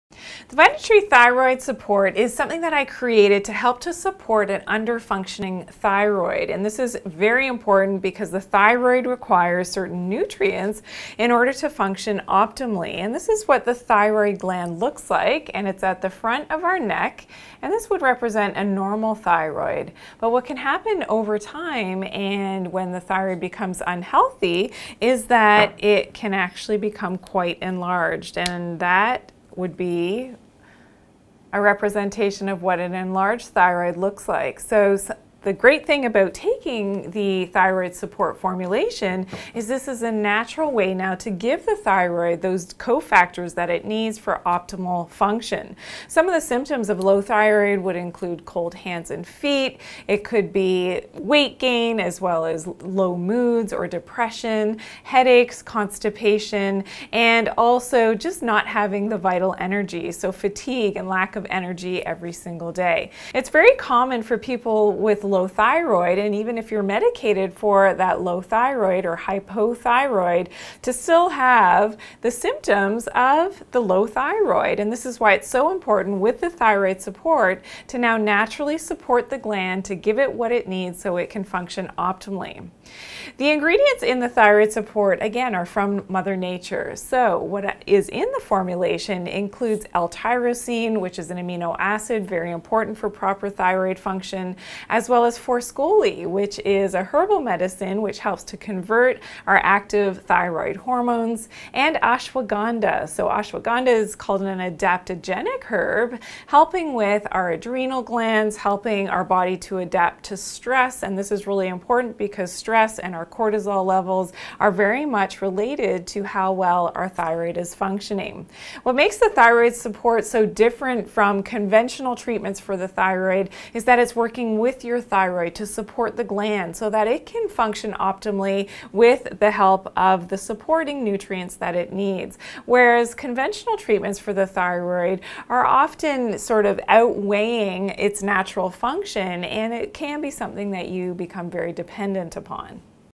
Vitatree Thyroid Support (Introduction)
VitaTree Thyroid Support is a natural whole food supplement for the support of an underactive thyroid gland. It gives nutritional support to the thyroid to correct an iodine deficiency and helps improve the proper functioning of the thyroid. VitaTree Thyroid Support is a highly absorbed whole food supplement to help support the health of the thyroid gland.  The thyroid gland is a very important regulator of the body. It helps to regulate the metabolism, energy levels, menstrual cycles, heart rate, respiratory rate, body temperature, muscle strength, cholesterol levels and much more. Low thyroid function is very common and called hypothyroidism.  Hypothyroidism is caused by a number of factors including; autoimmune attack of the thyroid or Hashimoto’s thyroiditis, bacterial and parasitic infections, stress, heavy metal toxicity, chlorine, fluoride and bromine toxicity, iodine deficiency, postpartum and even whiplash.

• 100% pure whole food supplement
• No magnesium stearate, artificial additives, fillers or colours
• GMP certified (Good Manufacturing Practice)

*To learn more about natural health, watch Dr. Janine Show @Dr. Janine Bowring, ND – Online every Monday 12pm ETS – And chat with Dr. Janine live during the show.